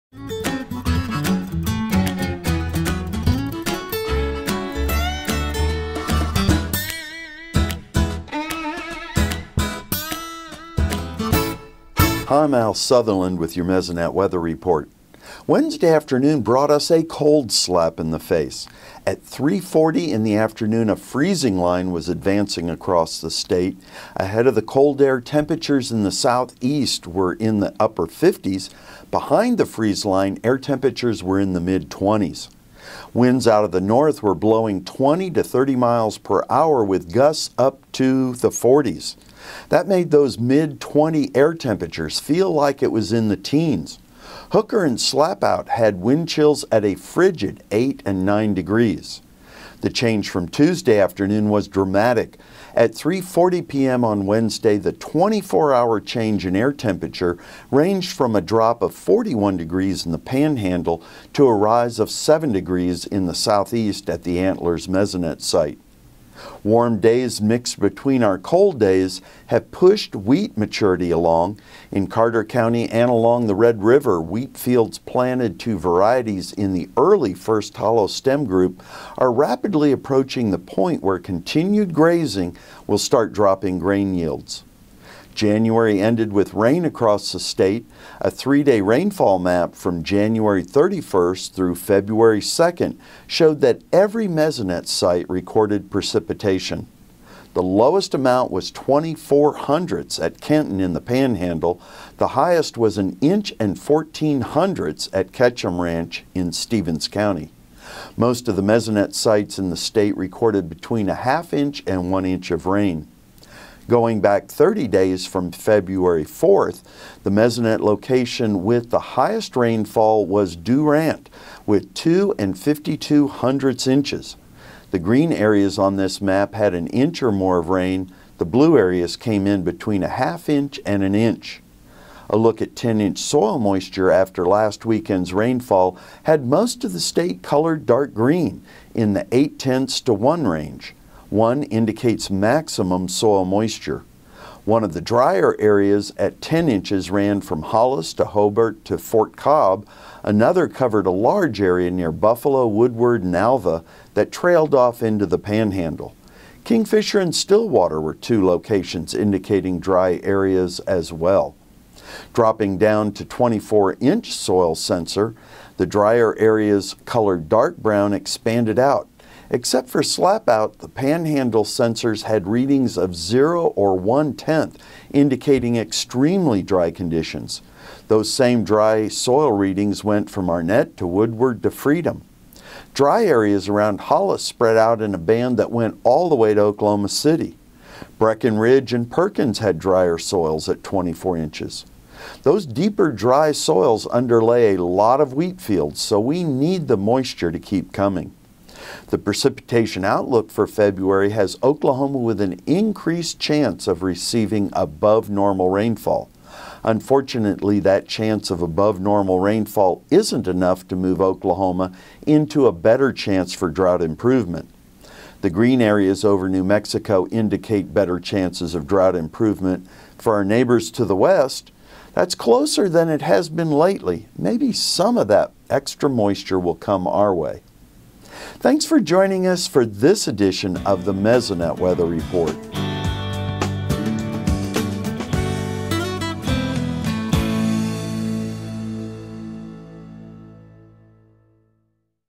Mesonet Weather (2/07/15)
Al Sutherland covers the wind speeds and temperatures from this week’s cold front, first hollow stem progress and 30-day rainfall amounts.